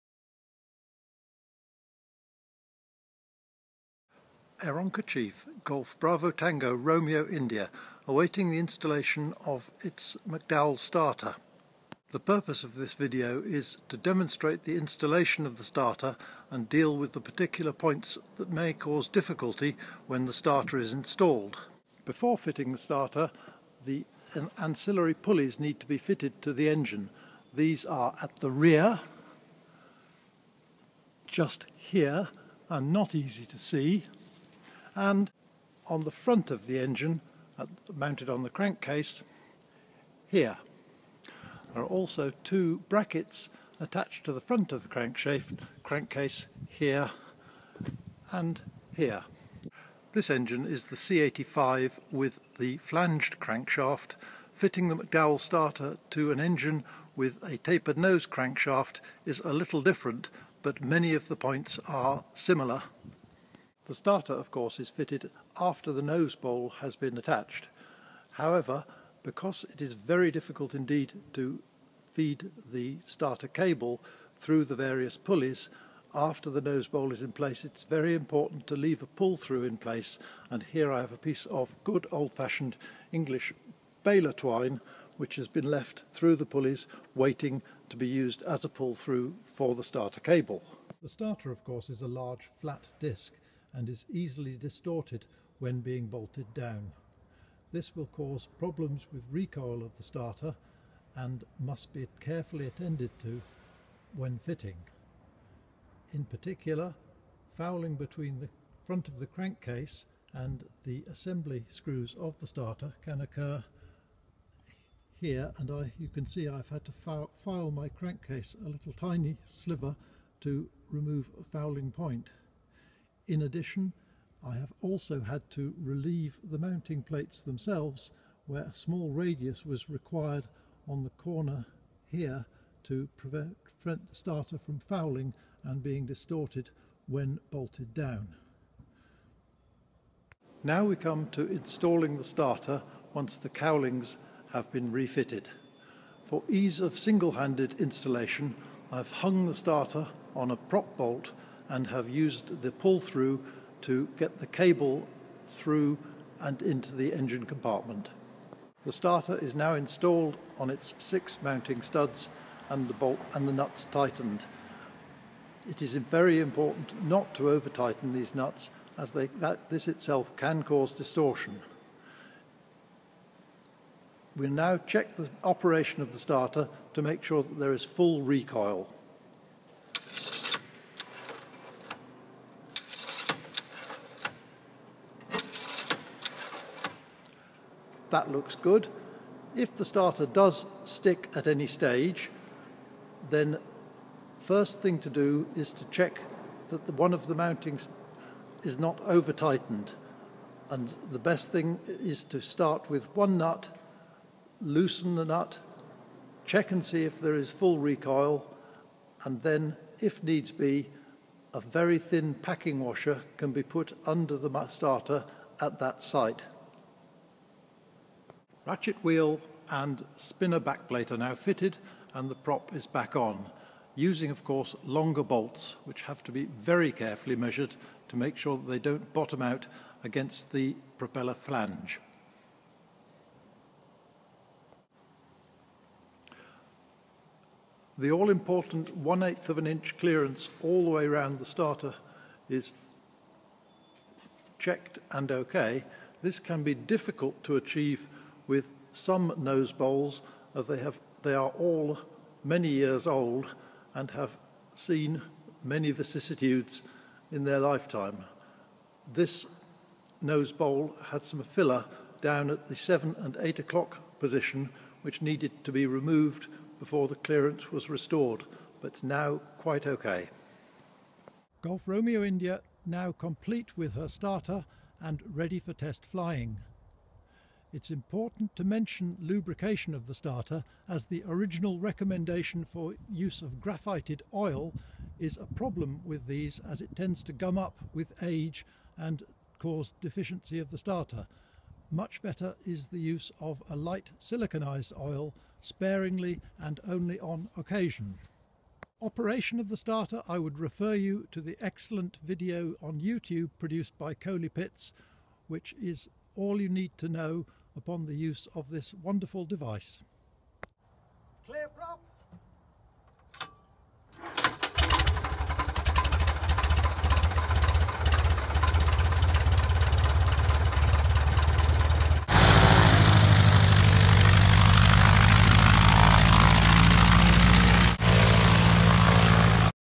Fitting a McDowell Starter to an Aeronca Chief
How to fit a McDowell Safety Starter to an Aeronca Chief light aircraft